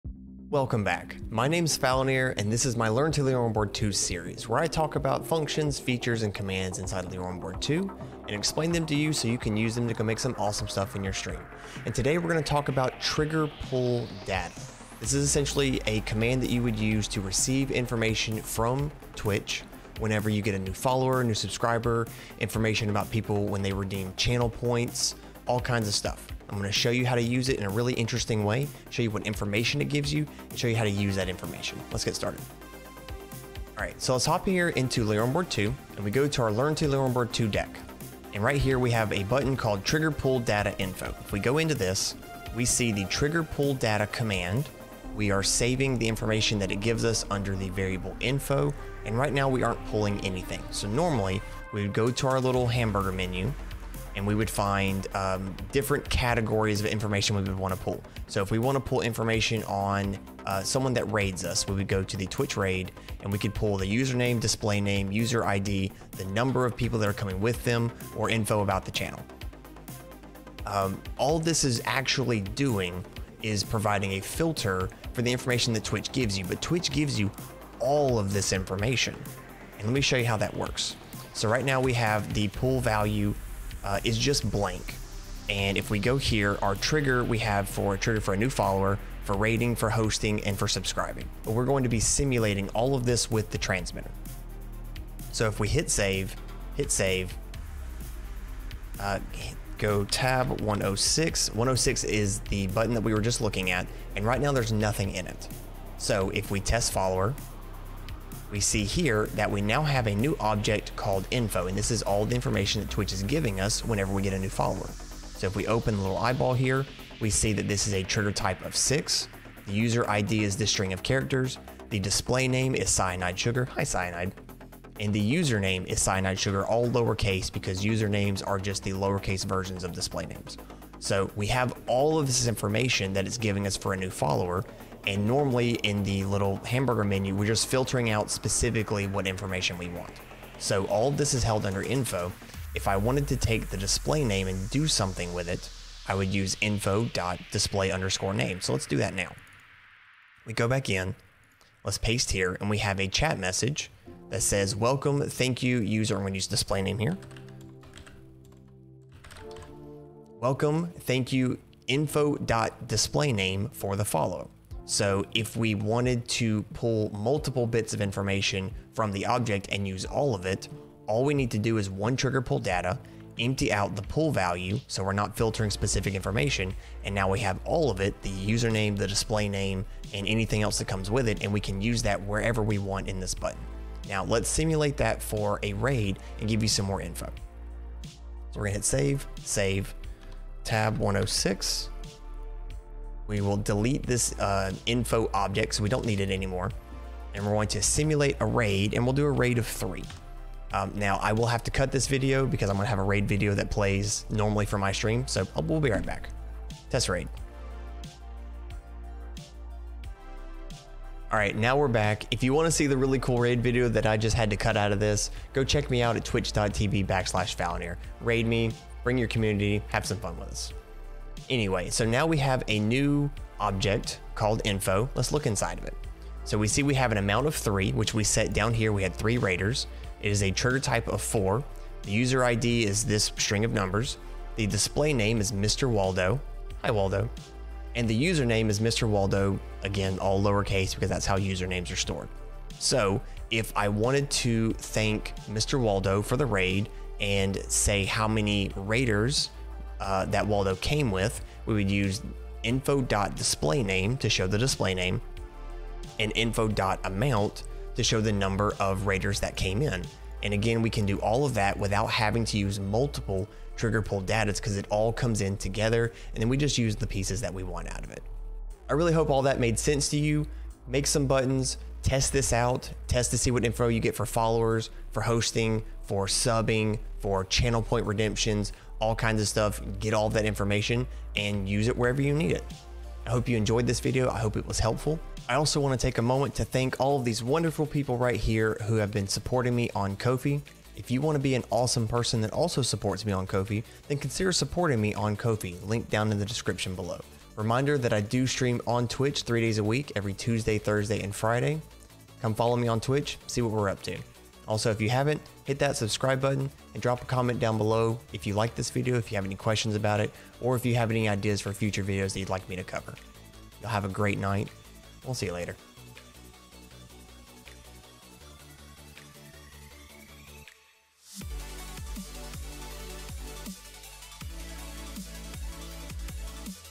Streaming with SAMMI - Trigger Pull Data
The next video in our series, Learn to Lioranboard 2, where we teach you how to use basic features in LB2 so you can start making awesome things! This video goes over the Trigger Pull Data command and how you can use it to get information from Twitch when a Twitch Event happens (new follower/subscriber/raid/host/etc). If you have any questions about this topic then hit me up in the comments or drop into our discord!

LINKS BELOW